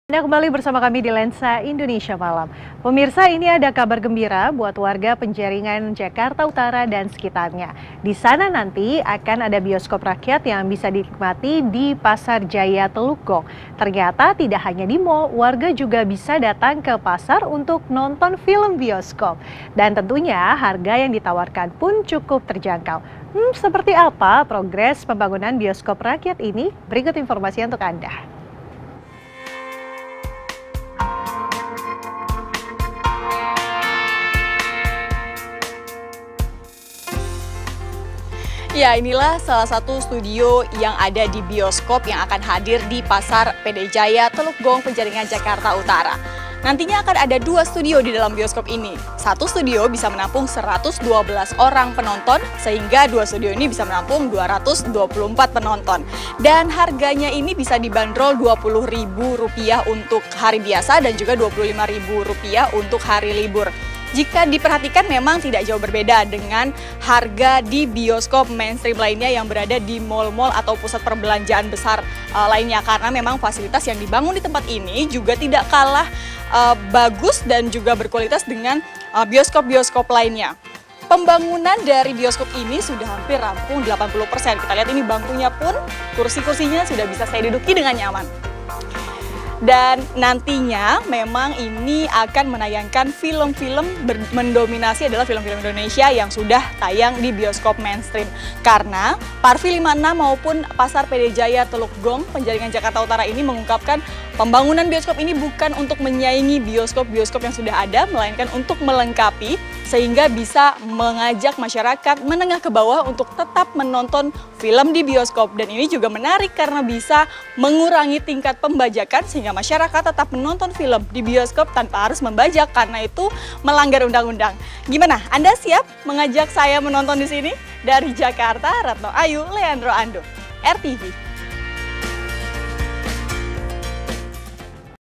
Kabar Gembira! Ada Bioskop Rakyat di Pasar Jaya Teluk Gong, Jakut
Sebuah  bisokop rakyat tengah dibangun di Pasar Jaya Teluk Gong, Penjaringan, Jakarta Utara. Warga dapat menonton film dengan harga terjangkau.